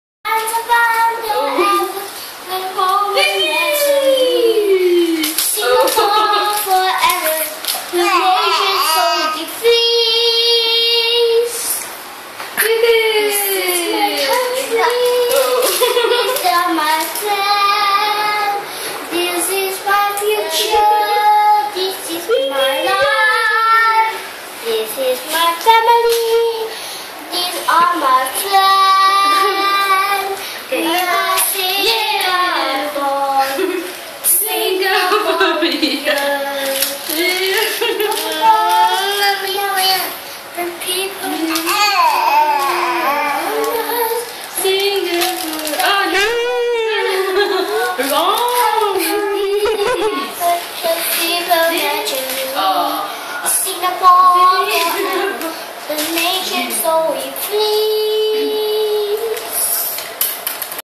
Yongle first National Day 2012
Going to mini parade at Jurong West Stadium 04-Aug-12 (Sat)